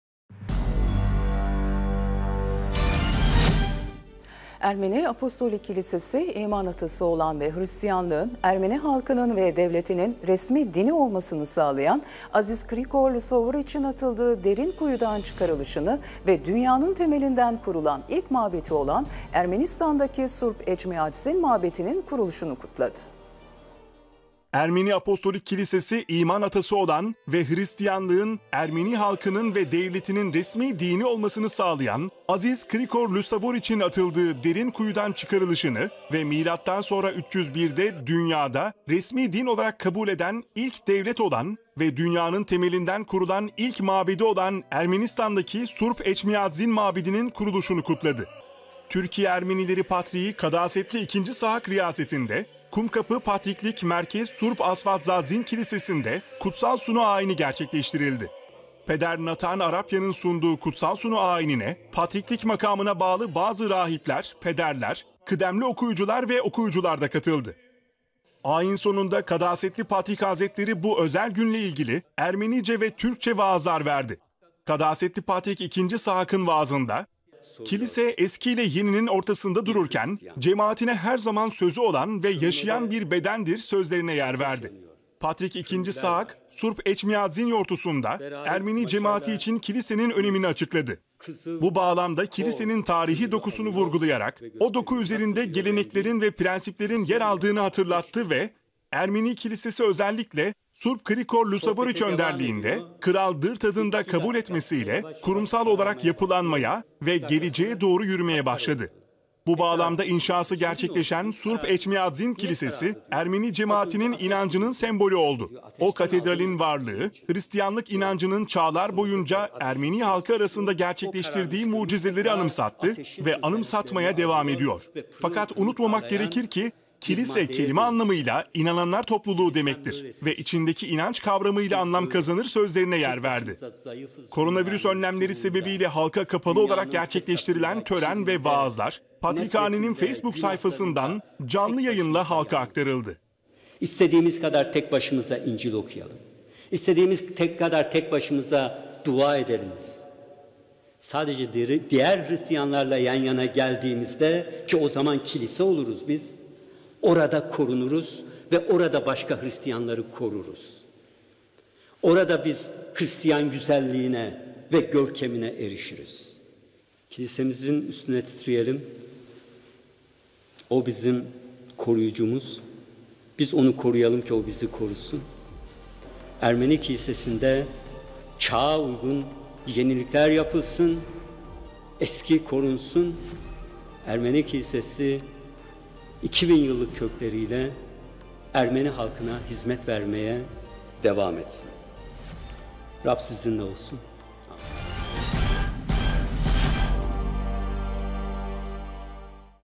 Ermeni Apostolik Kilisesi’nde İki Kutlama Birden Gerçekleşti | 19.06.2020
Ermeni Apostolik Kilisesi, İman Atası olan ve Hristiyanlığın, Ermeni halkının ve Devletinin resmi dini olmasını sağlayan Aziz Krikor Lusavoriç’in atıldığı derin kuyudan çıkarılışını ve M.S. 301’de Dünyada, resmi din olarak kabul eden ilk devlet olan ve Dünyanın temelinden kurulan ilk mabedi olan Ermenistan’daki Surp Eçmiadzin Mabedinin kuruluşunu kutladı.
14 Haziran Pazar günü, Ermeni Apostolik Kilisesi, Surp Eçmiadzin Mabedinin Kuruluşunu ve Surp Krikor Lusavoriç yortusunu kutladı.

Türkiye Ermenileri Patriği Kadasetli Sahak II. Riyasetinde, Kumkapı Patriklik Merkez Surp Asdvadzadzin (Meryem Ana) Kilisesi’nde Kutsal Sunu ayini gerçekleştirildi. Peder Natan Arapyan’ın sunduğu Kutsal Sunu ayinine, patriklik makamına bağlı bazı rahipler, pederler, kıdemli okuyucular ve okuyucular da katıldı.
Ayin sonunda Kadasetli Patrik Hazretleri, bu özel gün ile ilgili Ermenice ve Türkçe vaazlar verdi.

Kadasetli Patrik Sahak II.’nin vaazında; “Kilise eskiyle yeninin ortasında dururken, cemaatine her zaman sözü olan ve yaşayan bir bedendir” sözlerine yer verdi.
Patrik Sahak II., Surp Eçmiadzin Yortusu’nda, Ermeni Cemaati için kilisenin önemini açıkladı. Bu bağlamda kilisenin tarihi dokusunu vurgulayarak, o doku üzerinde geleneklerin ve prensiplerin yer aldığını hatırlattı ve, “Ermeni Kilisesi özellikle, Surp Krikor Lusavoriç önderliğinde, Kral Drtad’ın da kabul etmesiyle, kurumsal olarak yapılanmaya ve geleceğe doğru yürümeye başladı. Bu bağlamda inşası gerçekleşen Surp Eçmiadzin Kilisesi, Ermeni cemaatinin inancının sembolü oldu. O katedralin varlığı, Hıristiyanlık inancının çağlar boyunca halkımız arasında gerçekleştirdiği mucizeleri anımsattı ve anımsatmaya devam ediyor. Fakat unutmamak gerekir ki, kilise kelime anlamıyla inananlar topluluğu demektir ve içindeki inanç kavramıyla anlam kazanır” sözlerine yer verdi.

Koronavirüs önlemleri sebebi ile halka kapalı olarak gerçekleştirilen tören ve vaazlar Patrikhane’nin Facebook sayfasından canlı yayın ile halka aktarıldı.
SURP KRİKOR LUSAVORİÇ’İN DERİN KUYUDAN ÇIKIŞI ile ilgili yayınlanan yazı şu şekilde:

”İsa Mesih’in iki öğrencisi Tateos ve Partoğomeos atalarımıza İncil’i vaaz ettiler. Atalarımız ilk olarak onların vaazlarıyla aydınlandılar ve İsa Mesih’i Kurtarıcı olarak kabul ettiler.

İlk aydınlatıcılarımız Tateos ve Partoğomeos sadece İncil’i vaaz etmediler. Kiliseyi kurdular ve hayatlarını imanları uğruna feda ettiler. Onlar, Resuli (Apostolik) Ermeni Kilisesi’nin temellerine harç oldular.

İsa Mesih’in bu iki öğrencilerini kurdukları kilise, zamanın Hristiyanlara karşı yapılan eza hareketleri nedeniyle varlığını yeraltında sürdürdü.

Atalarımız, Tanrı’nın inayetiyle gerçekleşmiş bir mucizenin tanıkları oldular. Surp (Aziz) Krikor Lusavoriç imanı uğruna katlandığı işkenceler sonrasında atıldığı derin kör kuyudan çıktı. Gerçekleşen bu mucize sonrasında İman Atamız ve İkinci Aydınlatıcımız Surp (Aziz) Krikor Lusavoriç’in duasıyla Kral Drtad yakalandığı hastalıktan kurtularak, şifa buldu.

Atalarımız İncil’in vaaz edilmesiyle aydınlandılar ve başta Kral Drtad ve saray erkanı olmak üzere vaftiz edildiler. Hristiyanlık devlet tarafından serbest ve resmi din olarak ilan edildi.

Surp Krikor Lusavoriç’in şefaati hepimizin üzerinde olsun.”